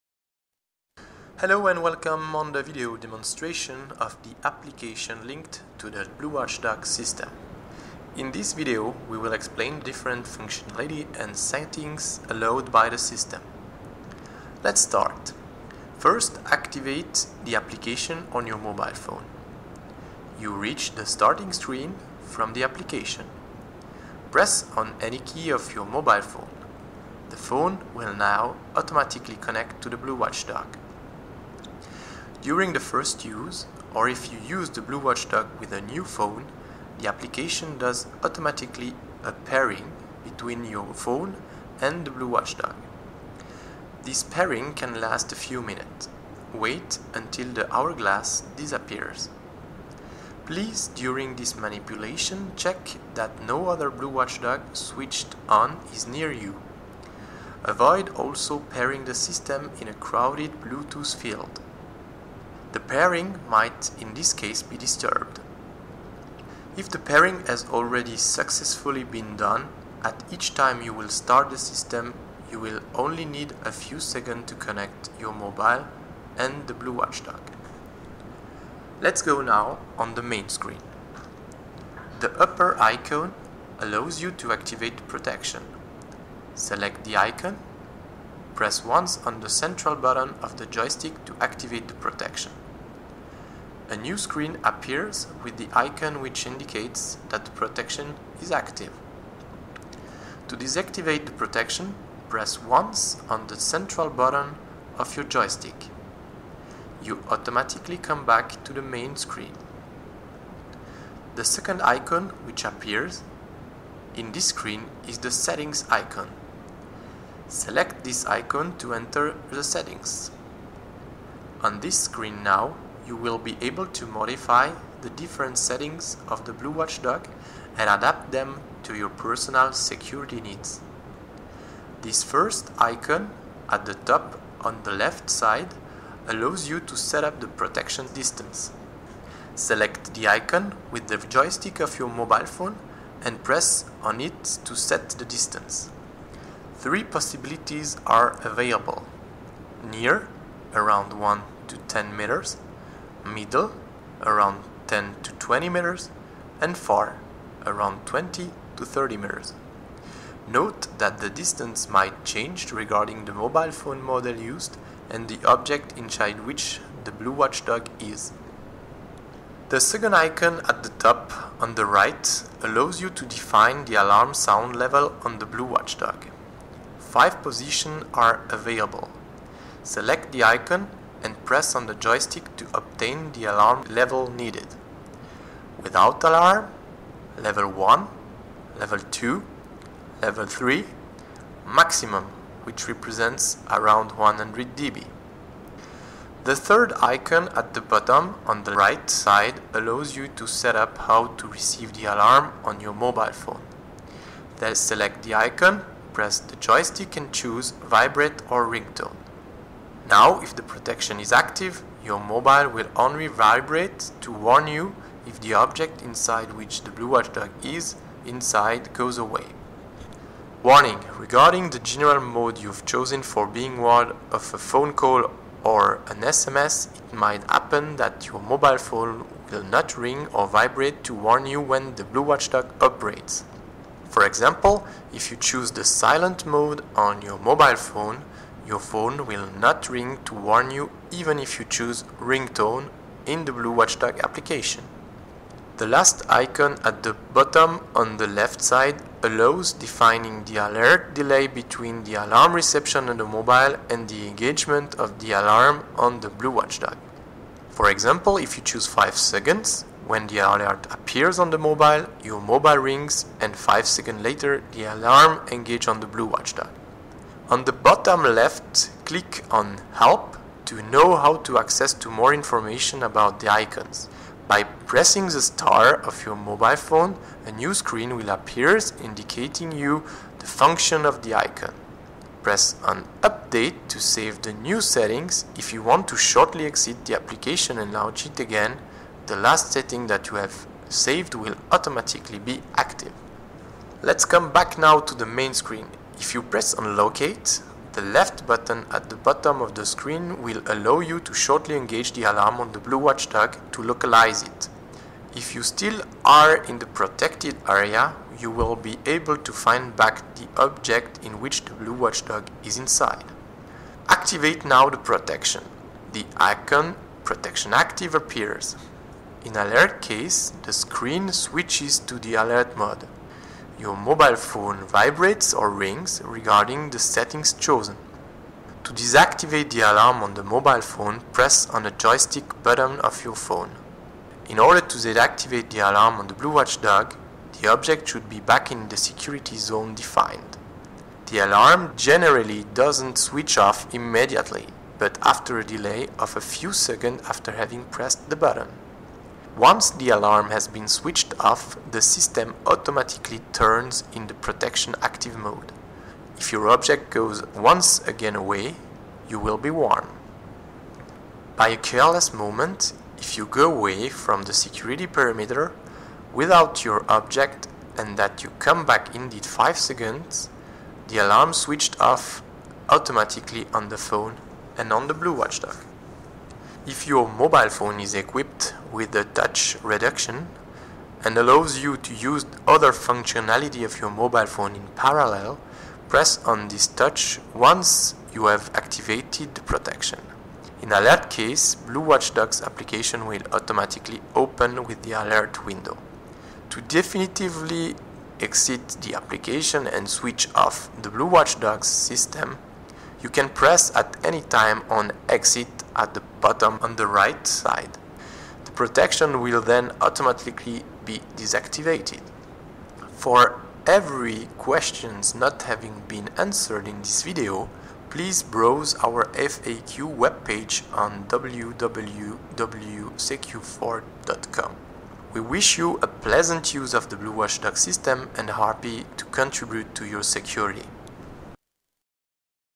BlueWatchDog Mobile Application Demonstration.avi
Everything you want to know about how the BlueWatchDog application works on your mobile phone is in this video.
Step after step you will be able to enter the detail of how the BlueWatchDog system will protect your valuables.